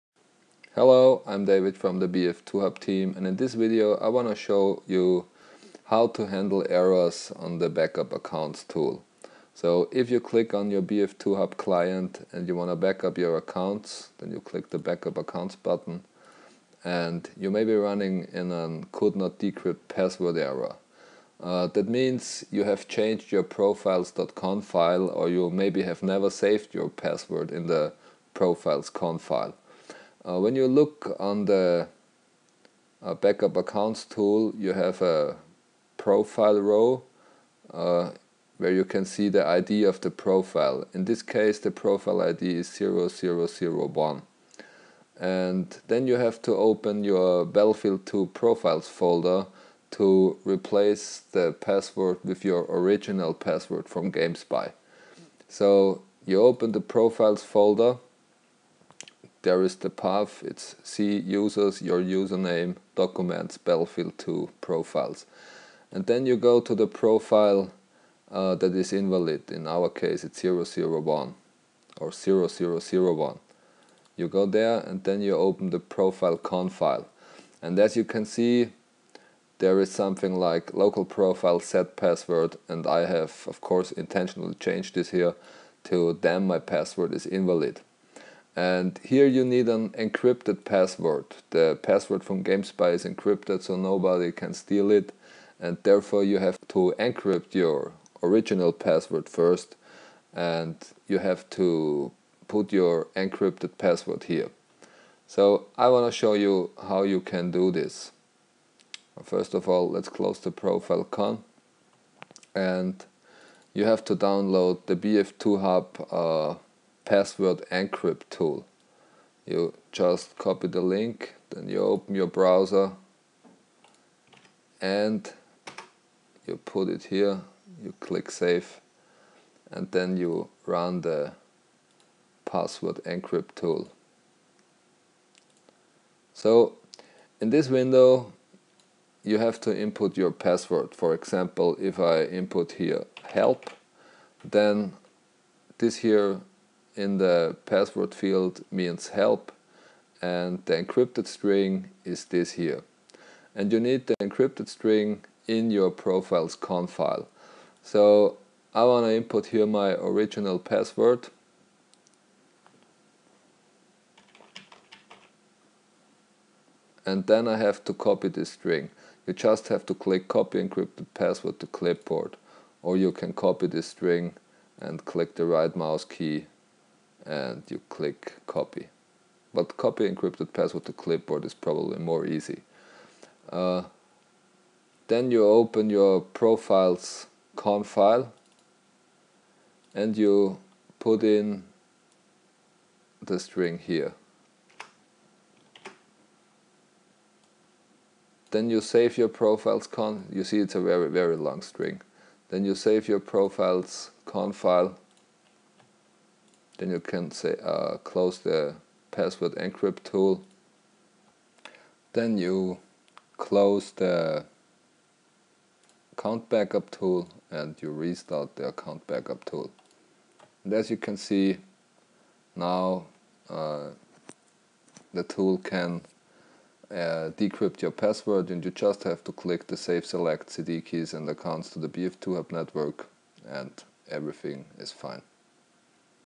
BF2Hub @ Fix Backup Account Errors
This Video will show you how to fix errors in your profile.con file.